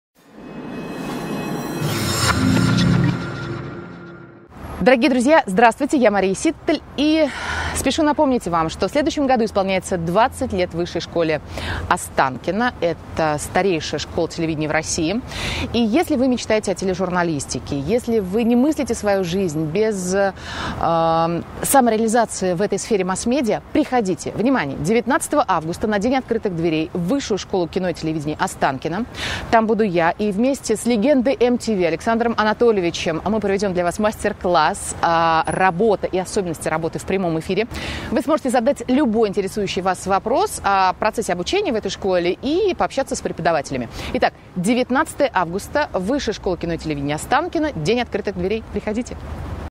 Мария Ситтель. Как стать телеведущим
Телеведущая и Мастер Высшей Школы «Останкино» Мария Ситтель приглашает на День Открытых Дверей, который состоится 19 августа.

Это уникальная возможность попробовать реализовать себя в таких профессиях, как: ведущий телевизионных и радио программ, журналист, редактор, режиссер, актер, гример, режиссёр монтажа, звукорежиссёр, оператор, продюсер и ивент-менеджер и стилист.

На Дне Открытых Дверей вы сможете посетить мастер-класс Марии Ситтель и Александра Анатольевича «Работа в прямом эфире», задать вопросы известным и успешным деятелям медиаиндустрии и попробовать себя в роли телеведущего.

МАРИЯ СИТТЕЛЬ – Мастер Высшей Школы «Останкино», телеведущая, журналист. Начала профессиональную карьеру в городе Пенза. С 2001 года работает в Москве на телеканале «Россия 1». Ведущая вечерних выпусков программы «Вести» и программы «Специальный корреспондент». Ведущая ежегодной специальной программы «Разговор с Владимиром Путиным». Многократный Лауреат премии ТЭФИ в номинации «Лучший ведущий информационной программы». Кавалер «Ордена дружбы» за большой вклад в развитие отечественного телевидения и многолетнюю плодотворную работу. Признана лучшей телеведущей по опросу журнала «7 дней» и «TNS Gallup Media».